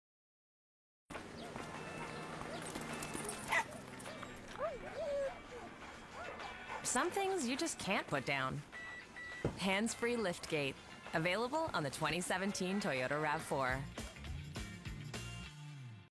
Toyota | Hands-Free Life Gate
Toyota has taken a unique step in its latest campaign to emphasize on its distinctive features in short videos instead of making one which is all inclusive. There is nothing special in this particular video but it works well for selling on their features.

Advertising Agency: Saatchi & Saatchi, Canada